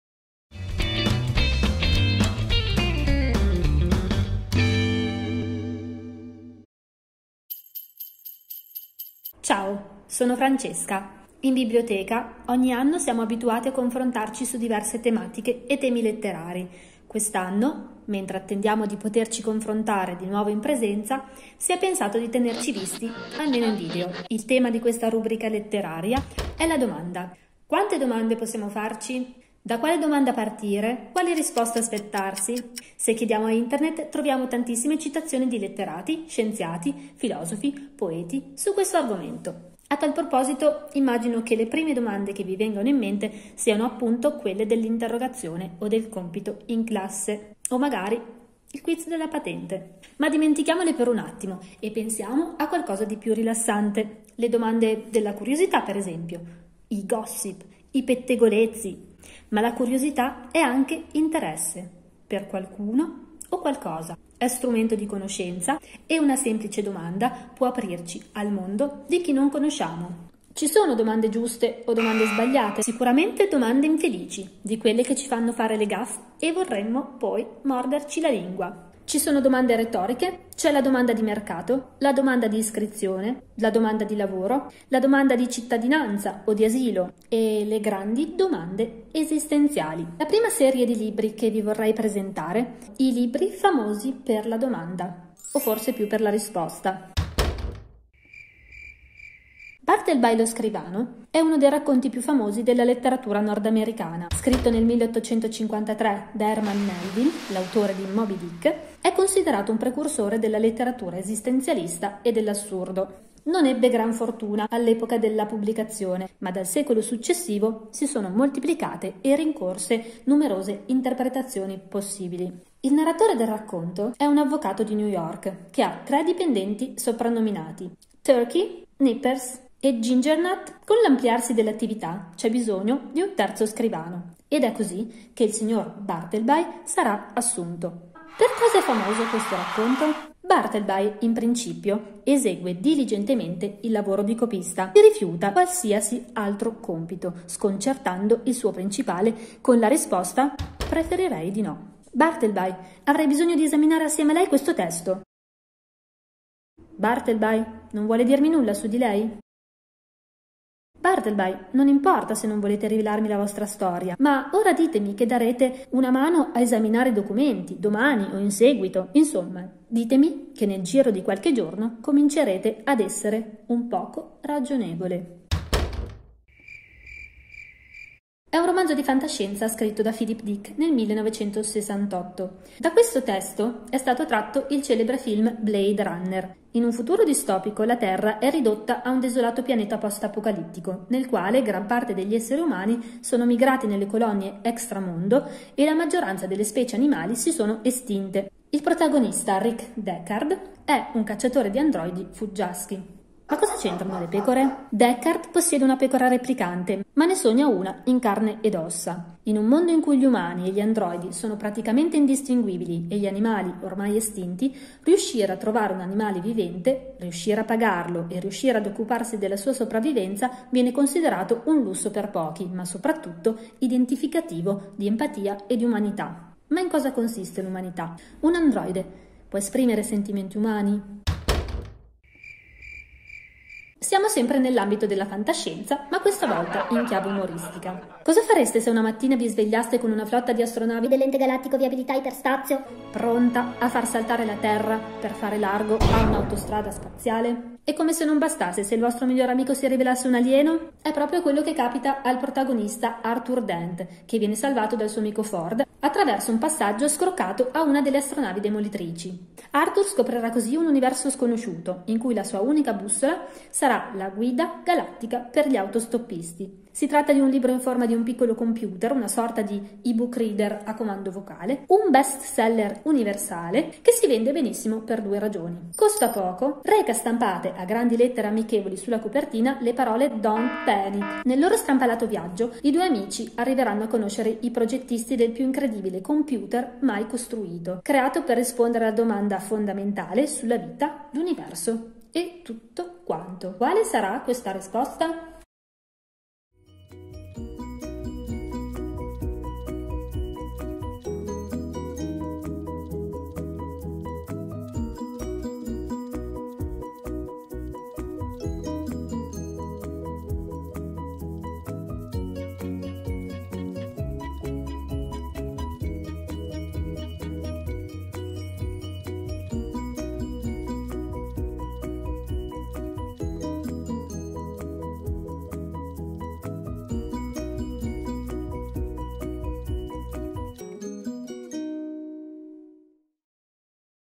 1. Dire fare domandare. Famosi per la domanda (o forse più per la risposta?)
Dire fare domandare è una rubrica letteraria per giovani studenti, pensata dallo staff della biblioteca Leoni per continuare i percorsi che normalmente si svolgono in presenza. Quest'anno gli incontri si sono tradotti in video.
Racchiusi nel tema della “domanda” ci sono diversi spunti che vanno dalla semplice curiosità fino ai rapporti interpersonali, passando per generi letterari diversissimi tra loro.

In questo video:
Bartleby lo scrivano di Herman Melville
Ma gli androidi sognano pecore elettriche? di Philip K. Dick
Guida galattica per gli autostoppisti di Douglas Adams